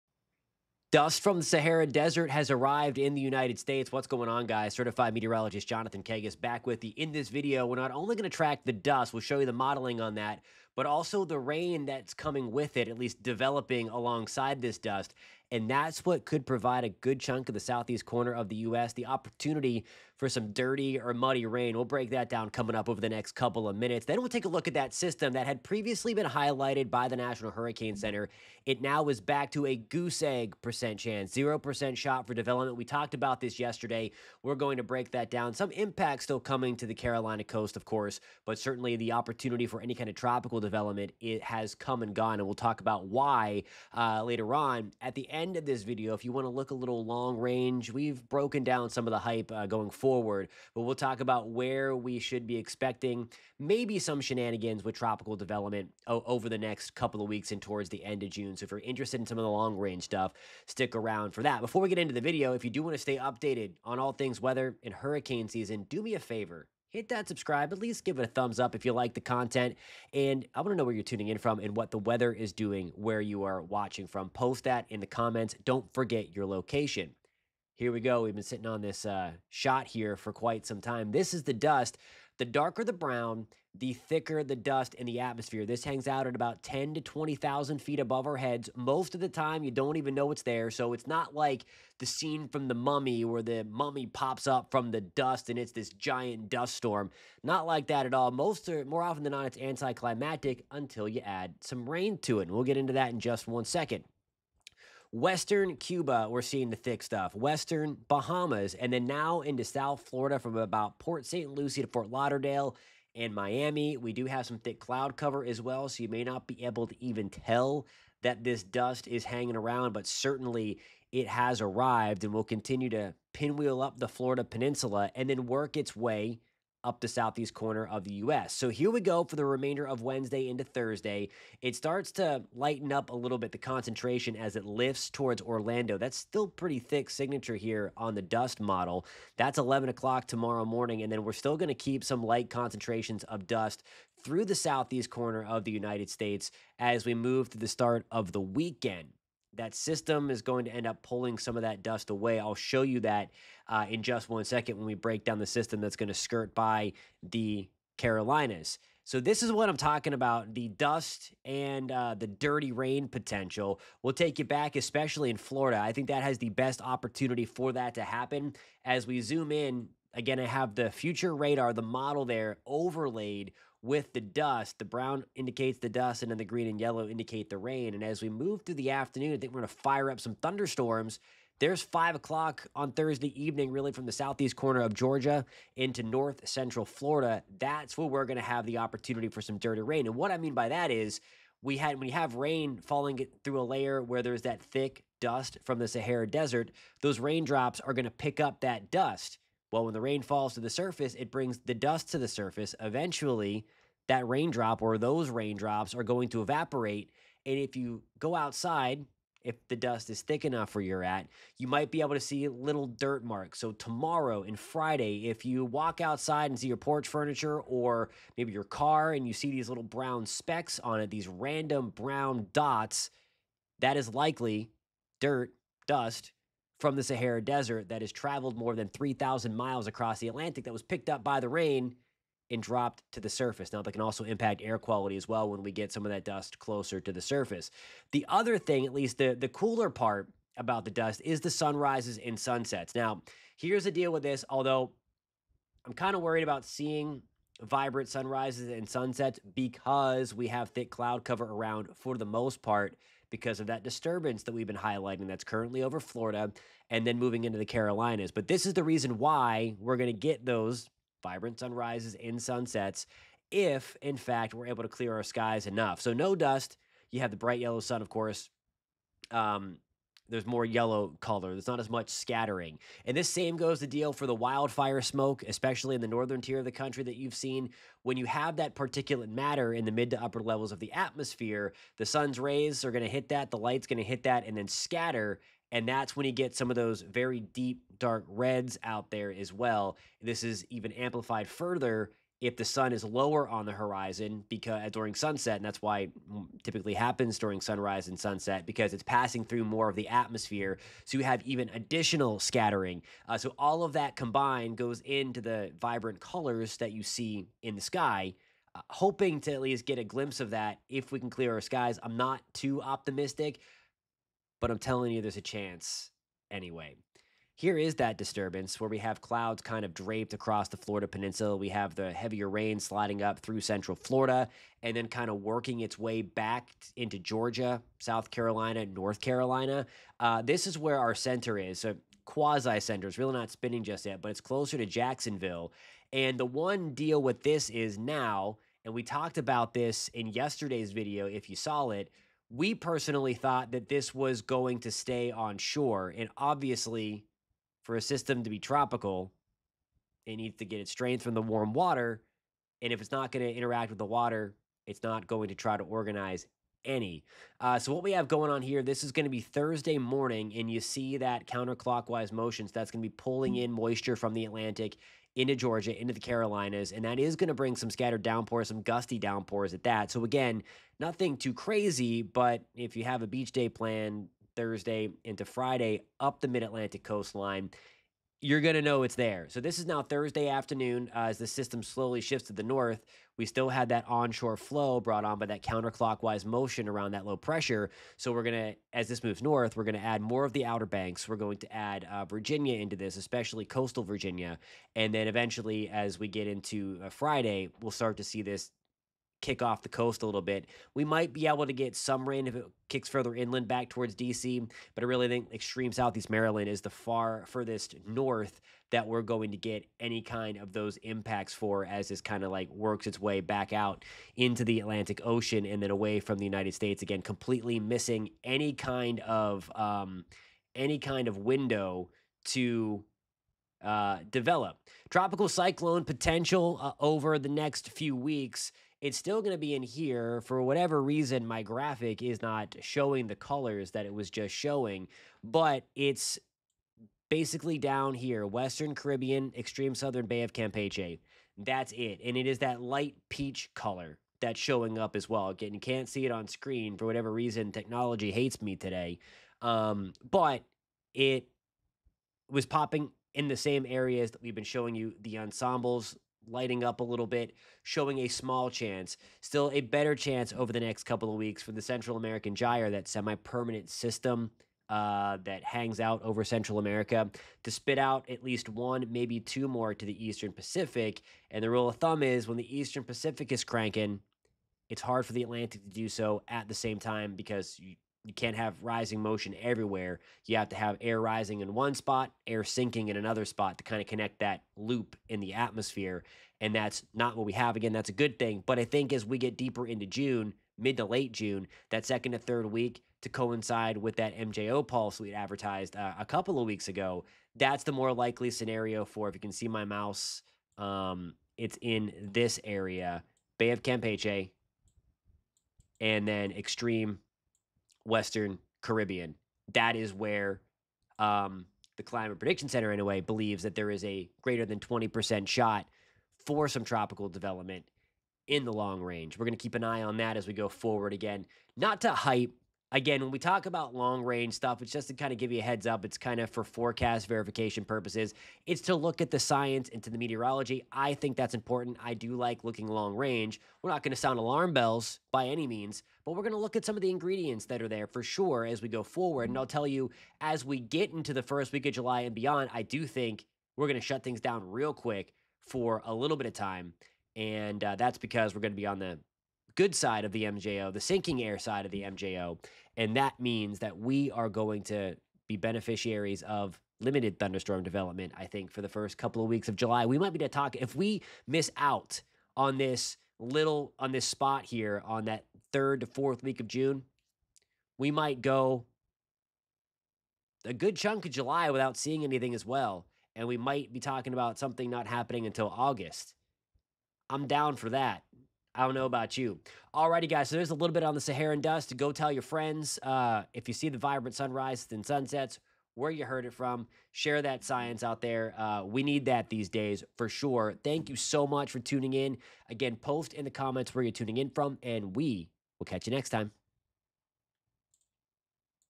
Big Plume Of Saharan Dust Arriving In The U.S. Could Make Rain Dirty (Tropics Update)
Saharan Dust, the yearly-occurring phenomena has made its trip across the Atlantic and has arrived in the U.S. This dust will combine with downpours to create dirty or muddy rain. As the rain falls, it picks up some dust, when the rain evaporates dirt marks are left behind on anything outside.

Chapters
00:00: Intro
1:15: Saharan Dust Arrives In U.S.
02:01: Saharan Dust Model
02:38: Dirty Rain Potential (Dust and Radar Model)
04:03: How Saharan Dust creates vibrant sunrises and sunsets
05:18: Update on disturbance being watched by the National Hurricane Center
05:47: Model For Carolina Storm
07:07: Long Range tropics outlook